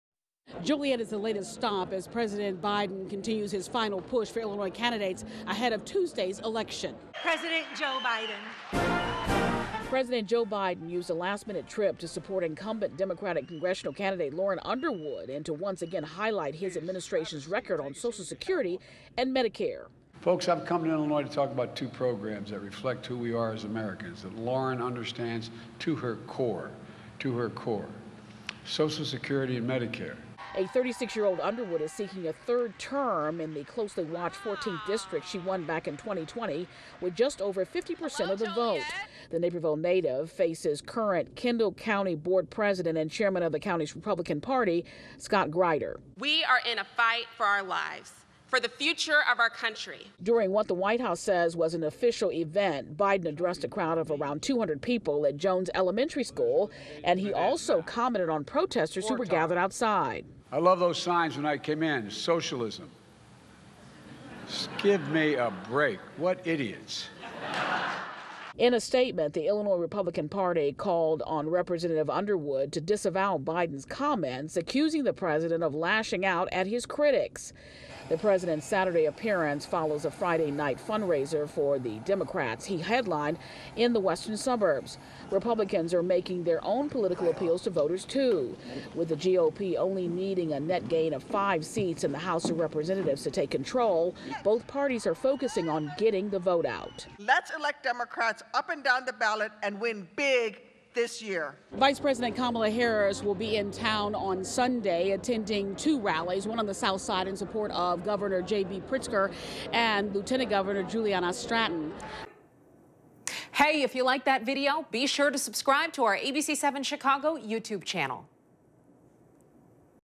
President Joe Biden calls protesters 'idiots' during Joliet visit in final midterm election push
In a statement, the Illinois Republican Party called on Rep. Underwood to disavow Biden's comment, accusing the president of lashing out at his critics.